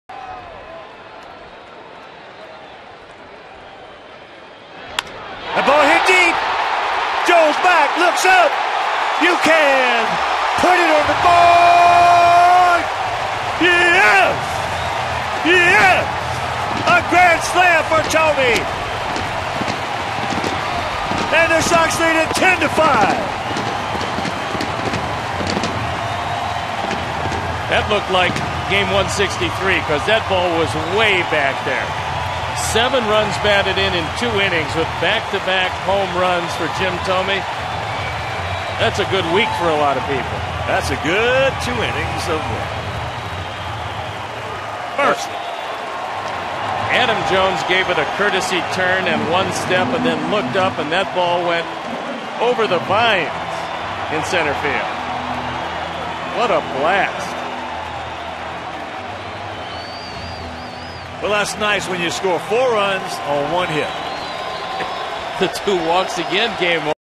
2009/07/17 Thome's grand slam
BAL@CWS: Thome smashes a grand slam to center field